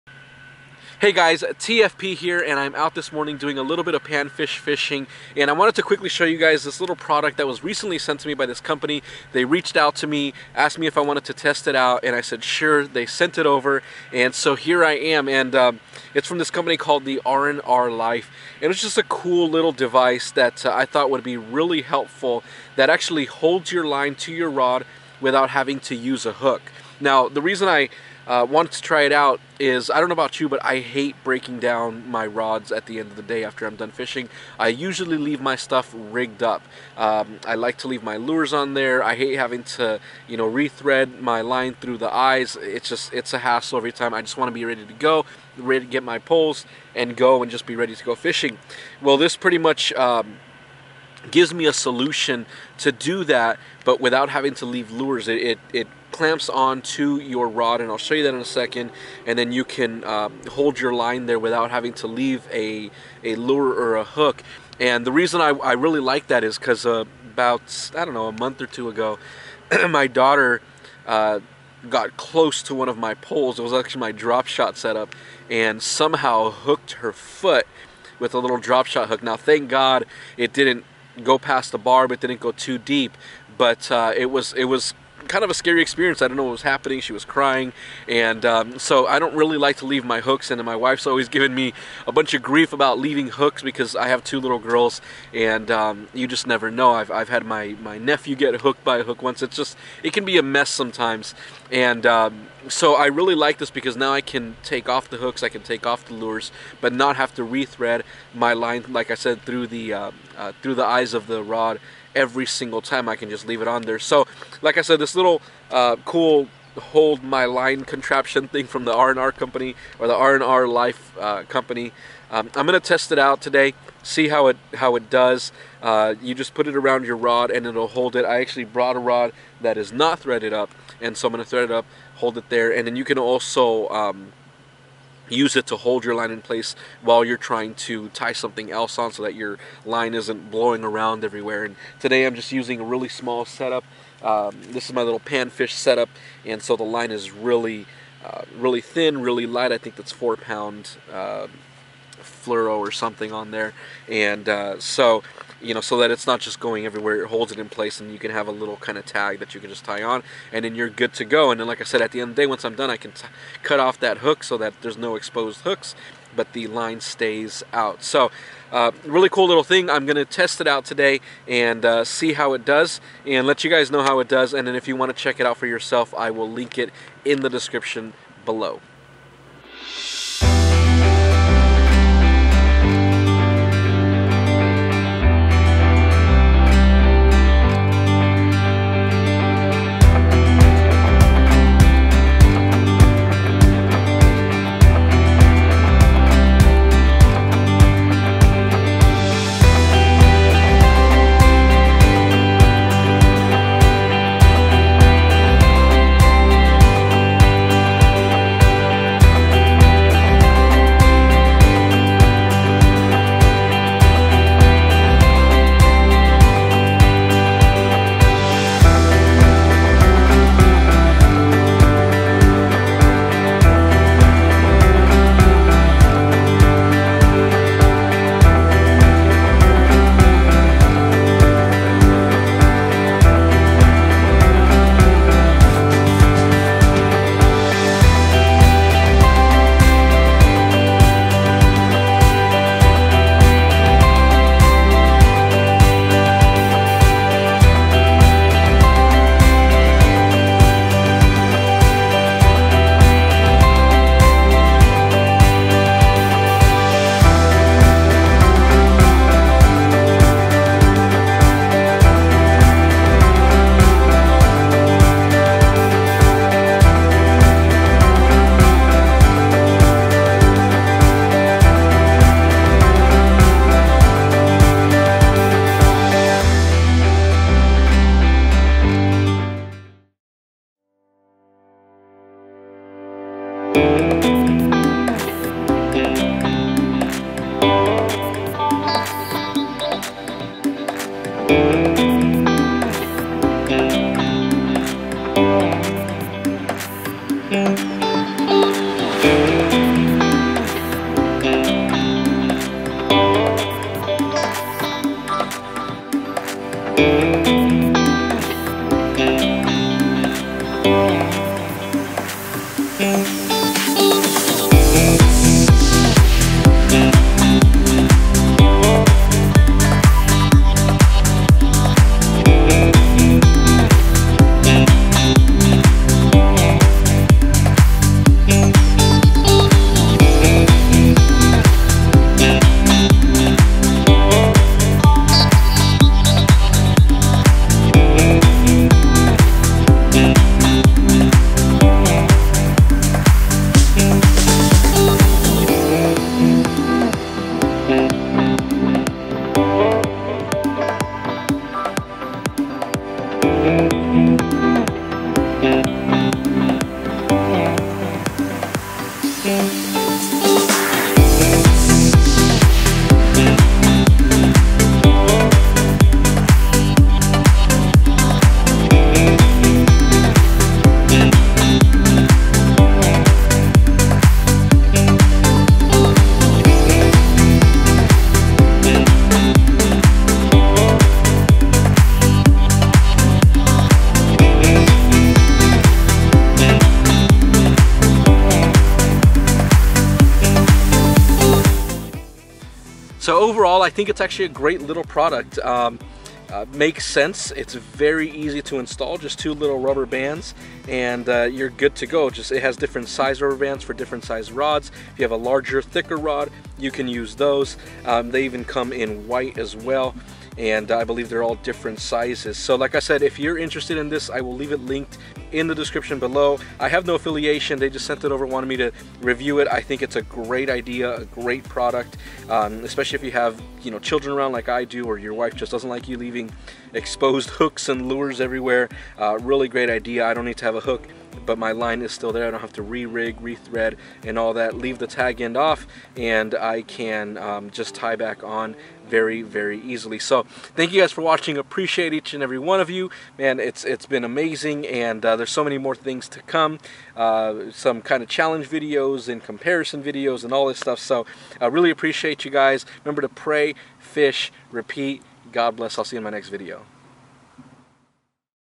R&R Life Product Review & Bluegill Fishing with Surprise Catch!
Hey everyone, sorry that I haven't posted a video for a while. Life has been very busy lately. I have tons of footage with many videos coming soon. Thanks for all of the support. God bless.

Pray. Fish. Repeat Clothing